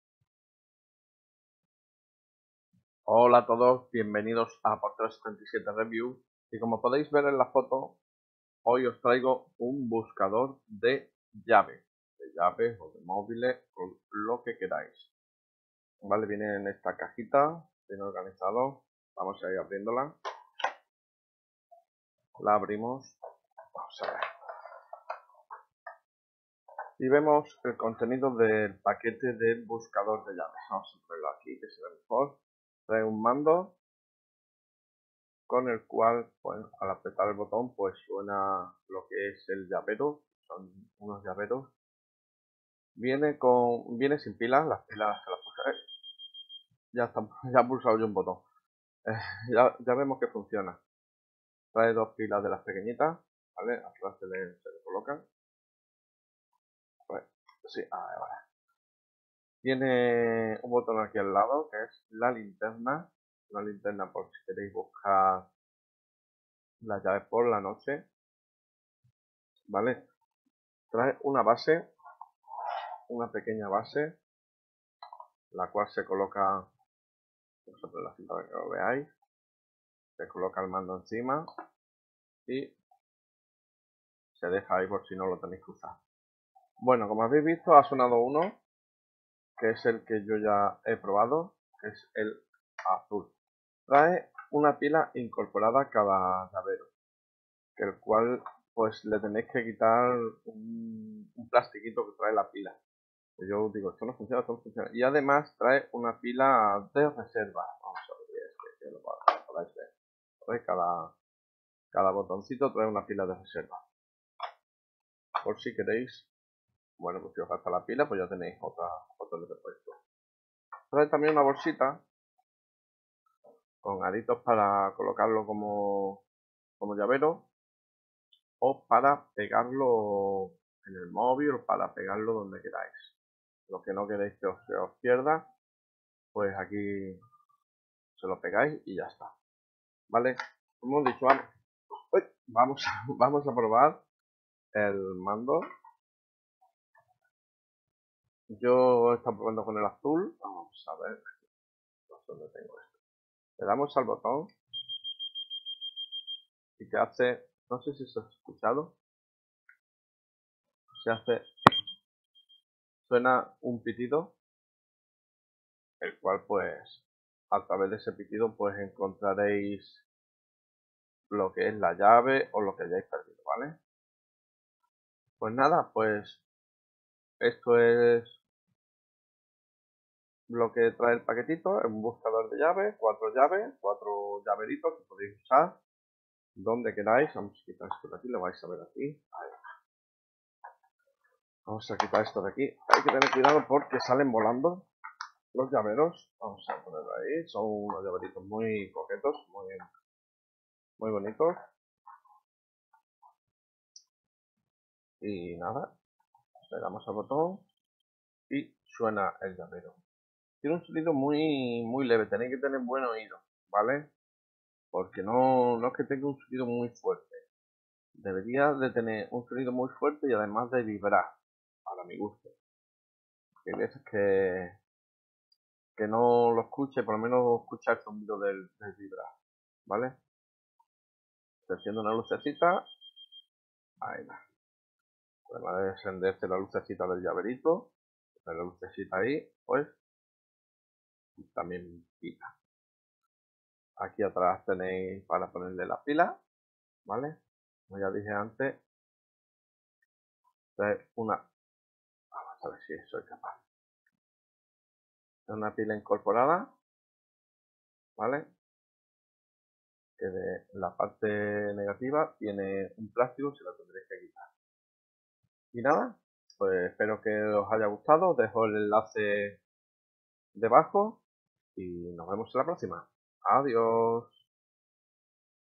review EIVOTOR Localizador de Llaves Inalámbrico con Base de Soporte
▶ La gama (hasta 40 m) le permite encontrar objetos de valor rápidamente en su hogar.

▶ Con linterna LED integrada, La frecuencia de radio puede penetrar la pared, el piso, la almohada y el cuero.

▶ Con pulsadores de gran tamaño, este buscador dellaves es fácil de operar y es especialmente adecuado para personas de la tercera edad.

▶ Busque fácilmente los elementos perdidos presionando los botones de color correspondientes. El pitido y las señales LED le guiarán para encontrar los elementos perdidos, y el receptor suena un pitido.

▶ No sólo obtendrá un buscador de llaves, sino también una linterna LED.